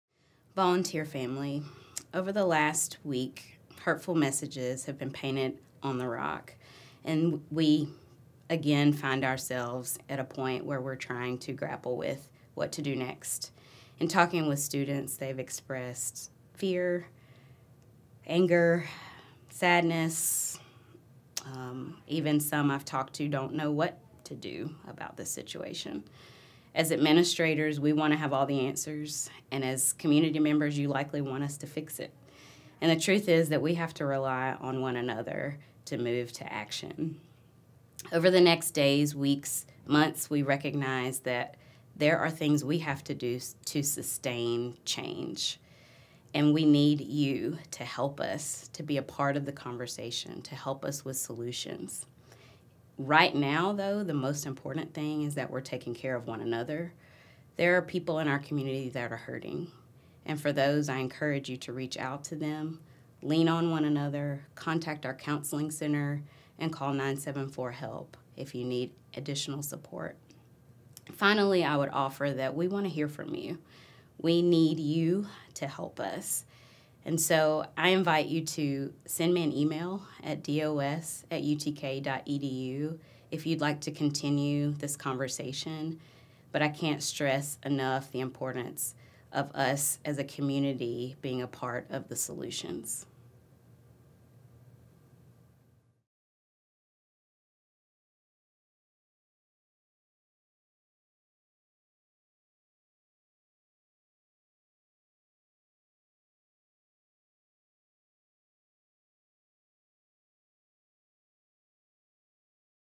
Vols Unite: Let’s Work Together
In light of recent threats against members of our community, it’s more important than ever to take care of each other and come together to work toward solutions.